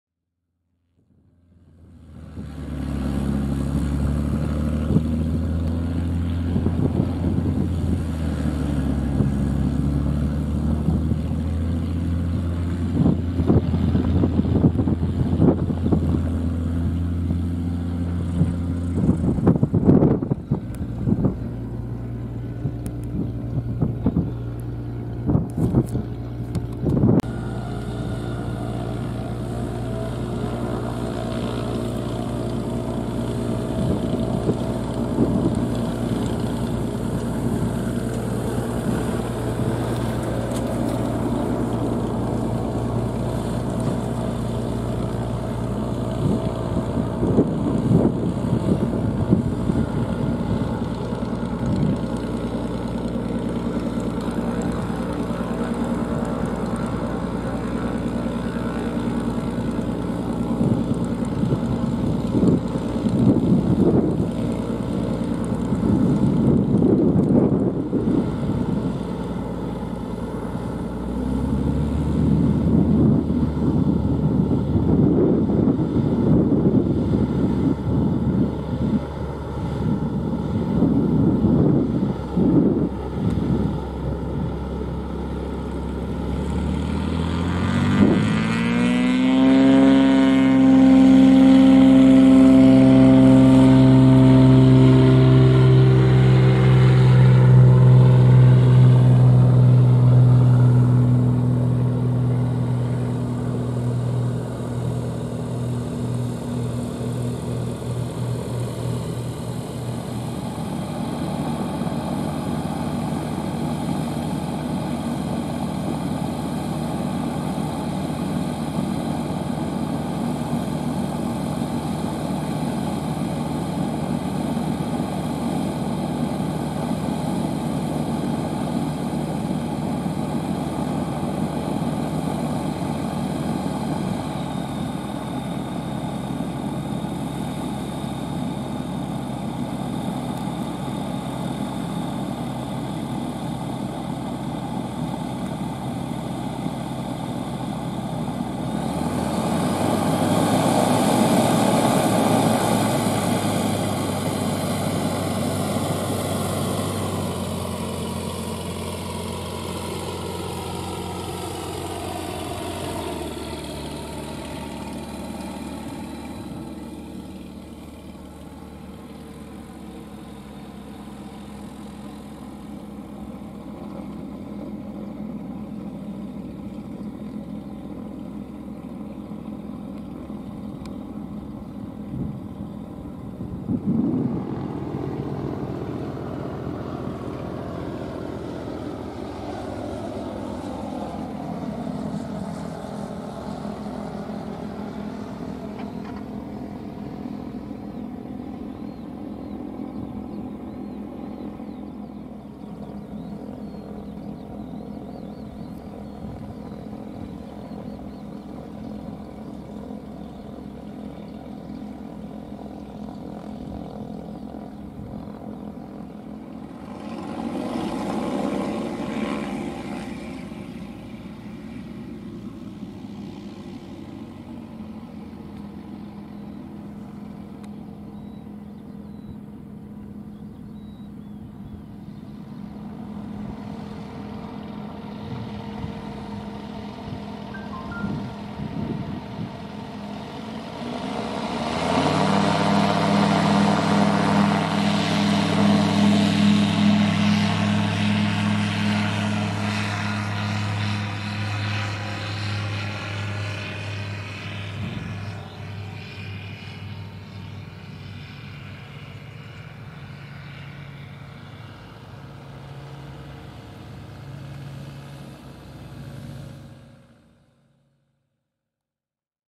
Start guest flight in an ultralight aircraft FK9
Start und Landungen auf der Piste 29 in EDKB Bonn-Hangelar.

*Micro-SD Karte
*=Afiliatelinks